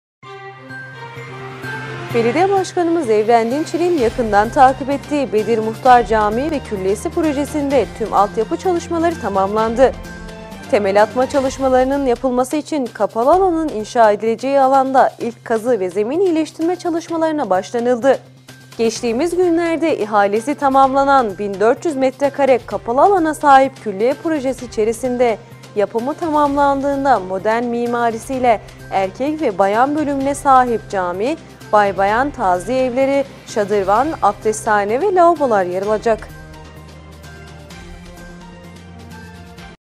BEDİR MUHTAR CAMİ VE KÜLLİYESİ PROJESİNDE İNŞAAT ÇALIŞMALARI BAŞLADI
Aksaray Belediyesi tarafından projelendirilen ve ihale aşaması tamamlanan Bedir Muhtar Cami ve Külliyesi Projesinde temel atmak için ilk kazı ve zemin iyileştirme çalışmaları başladı.

Belediye Başkanı Evren Dinçer’in yakından takip ettiği Bedir Muhtar Cami ve Külliyesi Projesinde tüm alt yapı çalışmaları tamamlandı. Temel atma çalışmasının yapılması için kapalı alanın inşa edileceği alanda ilk kazı ve zemin iyileştirme çalışmalarına başlanıldı.

Geçtiğimiz günlerde ihalesi tamamlanan bin 400 metrekare kapalı alana sahip Külliye projesi içerisinde yapımı tamamlandığında modern mimarisiyle erkek ve bayan bölümüne sahip Cami, bay bayan taziye evleri, şadırvan, abdesthane ve lavabolar yer alacak.

Bedir Muhtar Cami ve Külliyesi projesi ile ilgili açıklamalarda bulunan Belediye Başkanı Evren Dinçer, Külliye projesinin Bin 400 metrekarelik kapalı alanı kapsadığını ve proje sahasına külliye inşaatının yanında sosyal donatılar ve altyapı çevre düzenleme işlerinin yapılacağını belirtti.

Başkan Dinçer yaptığı açıklamada şu ifadelere yer verdi; ‘’Bedir Muhtar Mahallesinde Bedir Muhtar Kabristanlığı içerisine inşa ettiğimiz külliye projemizde erkek ve bayan bölümüne sahip Cami, bay bayan taziye evleri, bay bayan lavabolar ve abdesthaneler, şadırvan yer alıyor. Bedir Muhtar Mahallesinde tarihi dokuyu yaşatmak, eserlerin gelecek nesillere aktarımını sağlamak ve şehirleşme estetiği ile tarihi özellikleri aynı düzlemde buluşturmak maksadıyla çalışmalarına başladığımız Bedir Muhtar Külliyesinin inşaat çalışmaları tamamlandığında Aksaray için önemli tarihi mekân ortaya çıkmış olacak.’’ Dedi